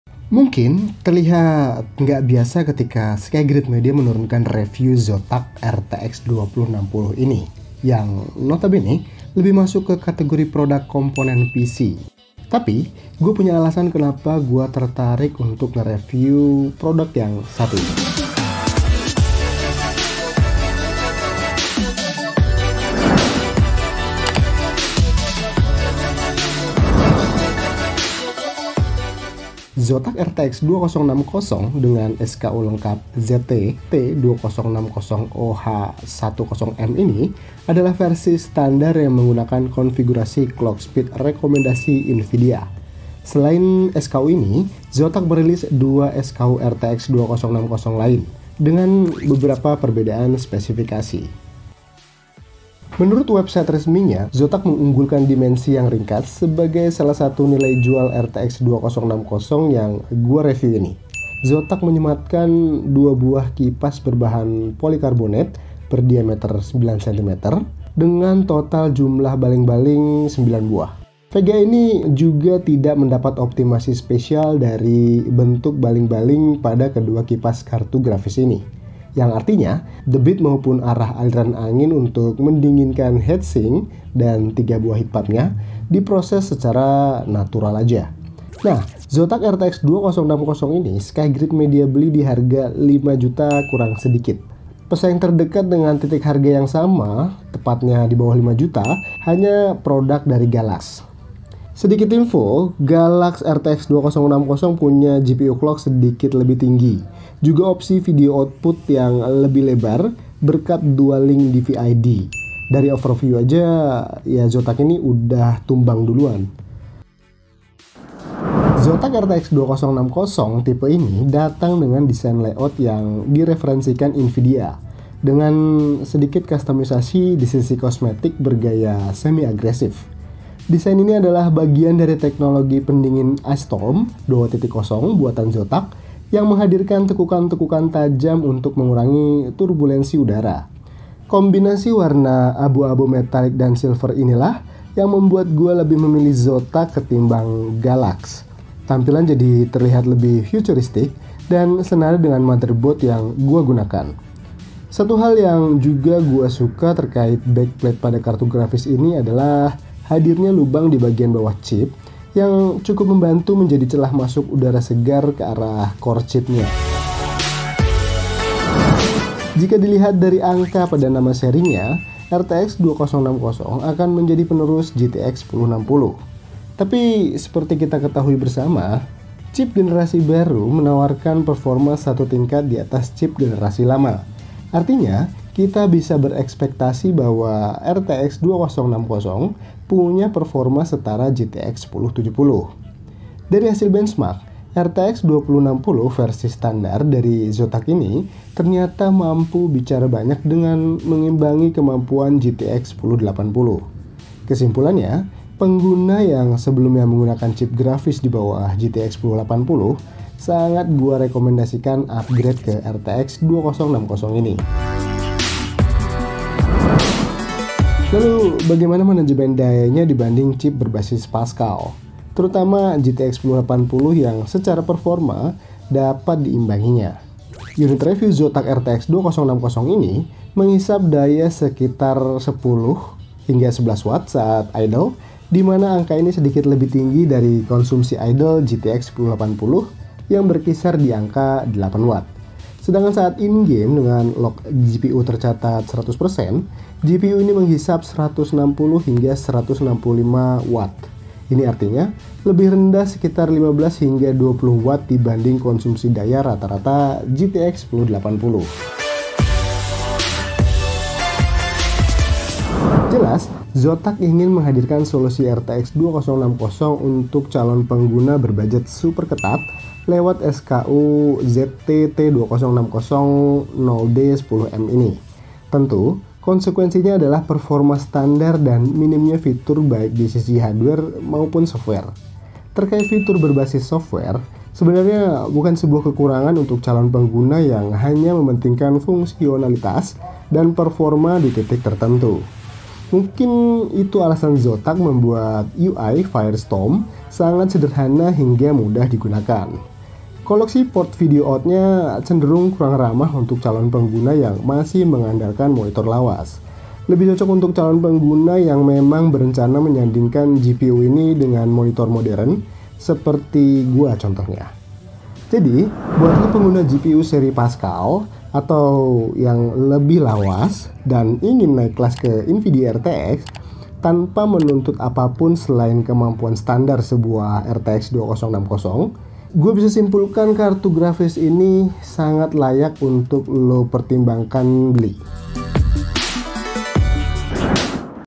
Review ZOTAC RTX 2060 - Kartu Grafis RTX Termurah Se-Indonesia
Unit review Zotac RTX 2060 ZT-T20600H-10M ini kita beli di harga 4,950,000,- yang membuat doi jadi VGA RTX termurah untuk saat ini.

Yang unik, Zotac RTX 2060 ini masih menyimpan nafas yang bisa membuatnya bersaing dengan RTX 2060 lain yang lebih mahal.
Selengkapnya, cek video review Zotac RTX 2060 ZT-T20600H-10M berikut.

Follow Soc-Med kita juga ya!
Facebook fanpage - SkyegridMediaID